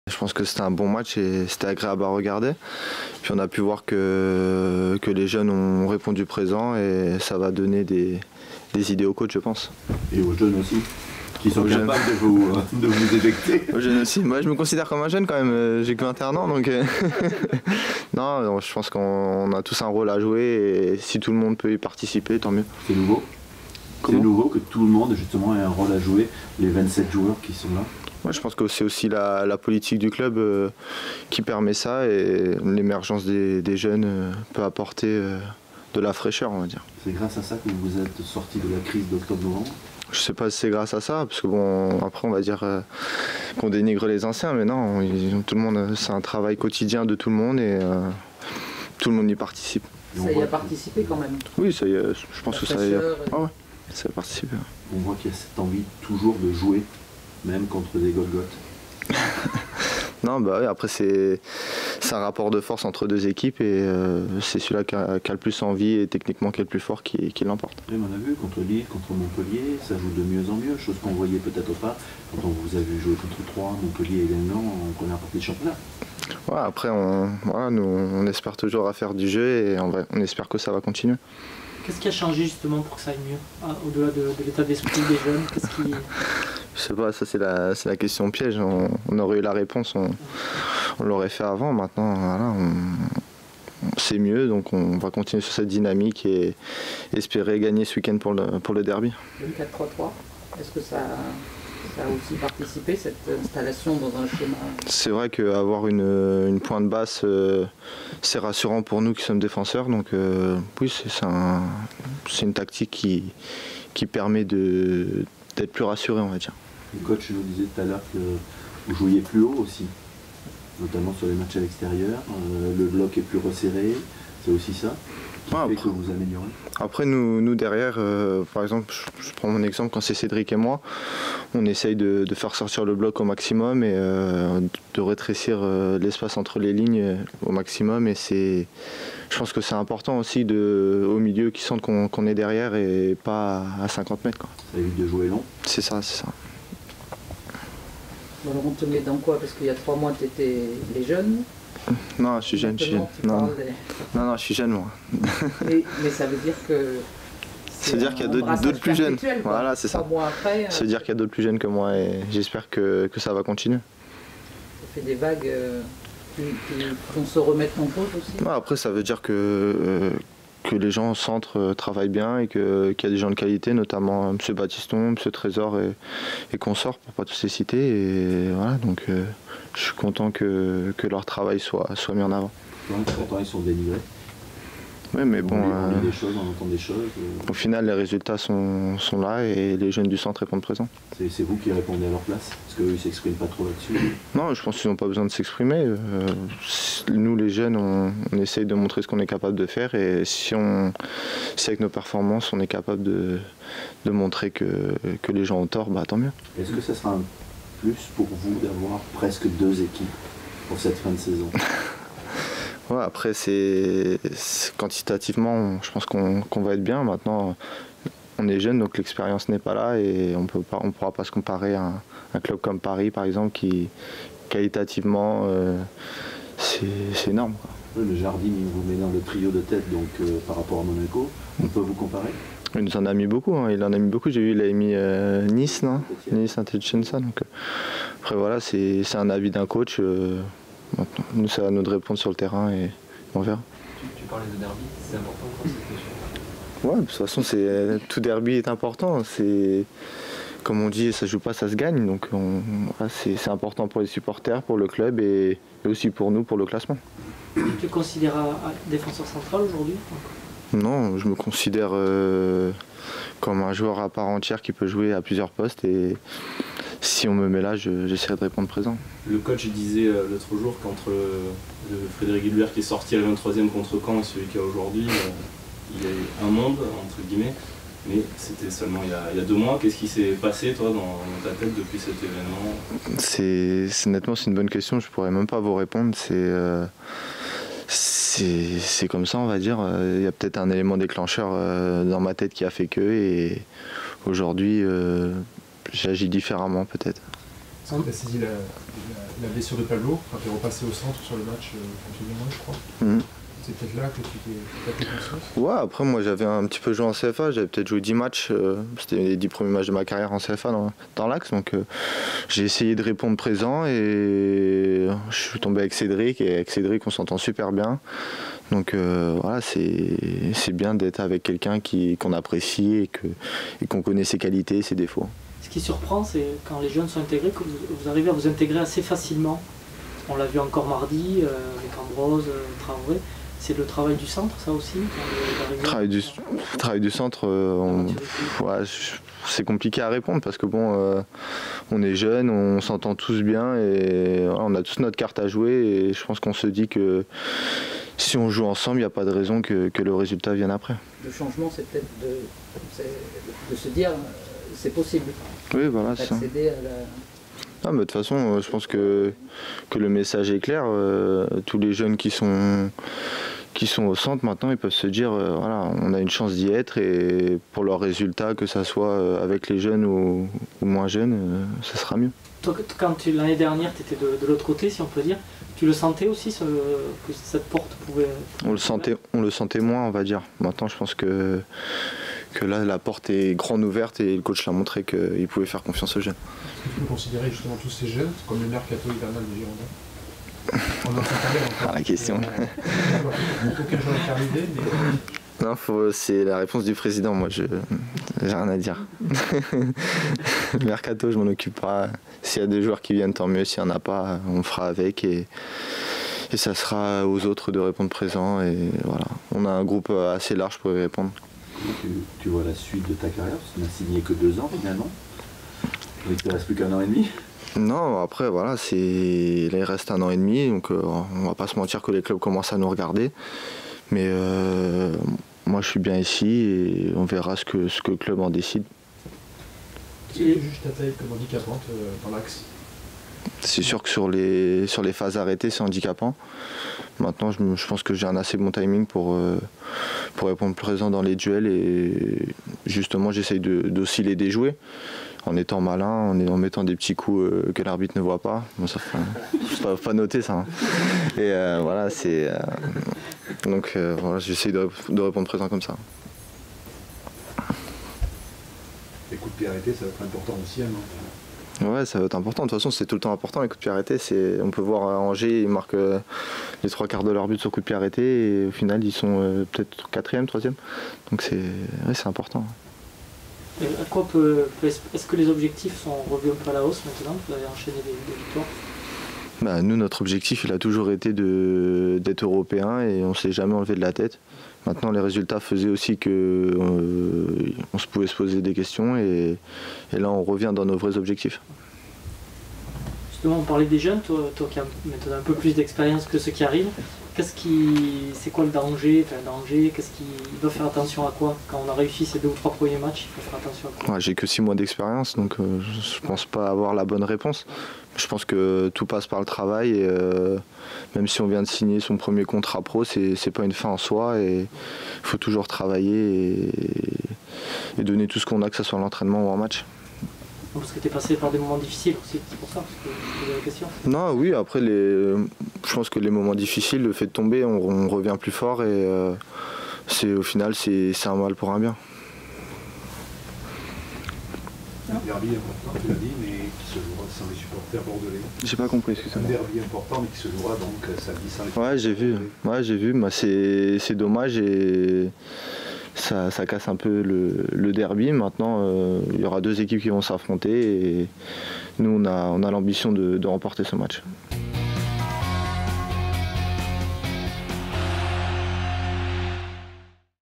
Conférence - Frédéric Guilbert avant Bordeaux/Nantes
Frédéric Guilbert est passé en conférence de presse d'avant-match jeudi. Retrouvez ses propos avant d'affronter Nantes !